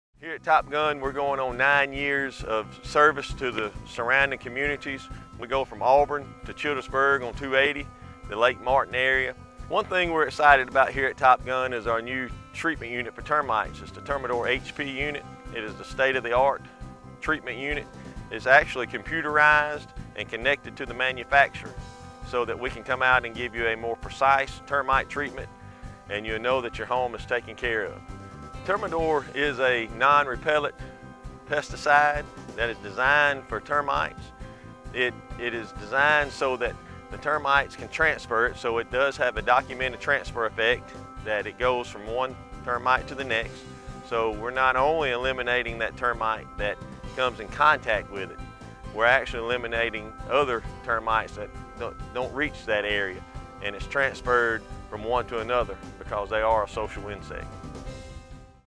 Top Gun Pest Control - Darren 1
Top Gun Pest Control owner, Darren Fuller, discusses servicing the Lake Martin, Auburn, Sylacauga, and surrounding areas for 9 years. Darren also discusses new upgrades for our termite protection service options.

Vido production needs?
We can help here at LMG!